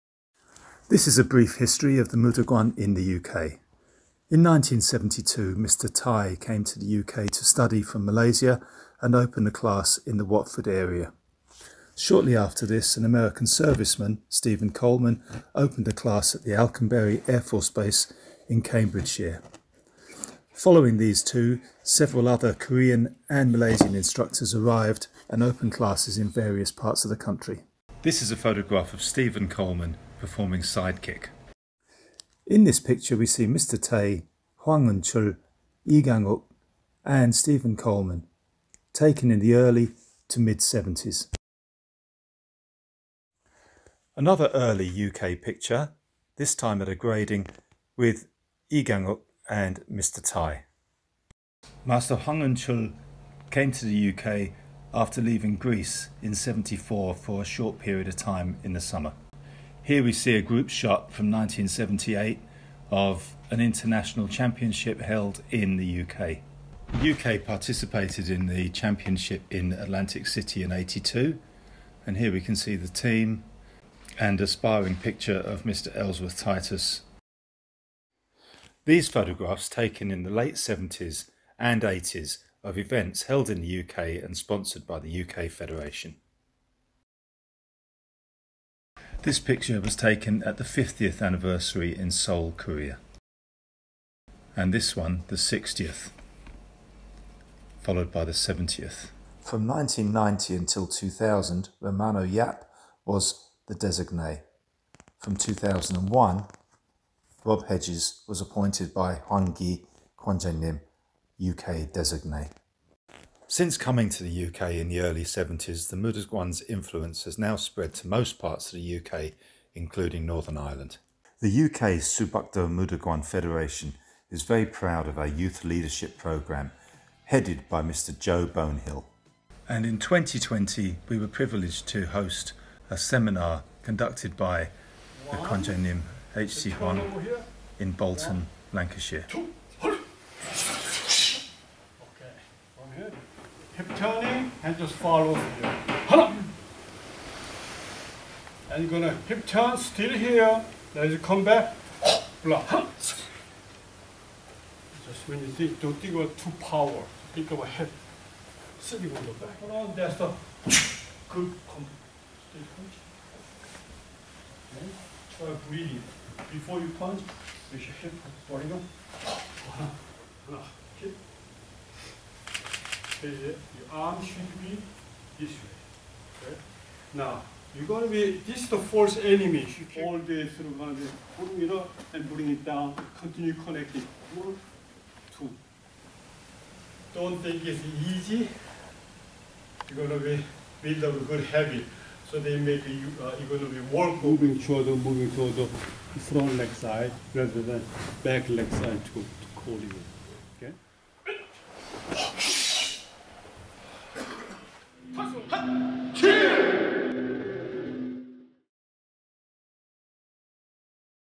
UK Moo Duk Kwan History Video
A brief history of the World Moo Duk kwan in the UK.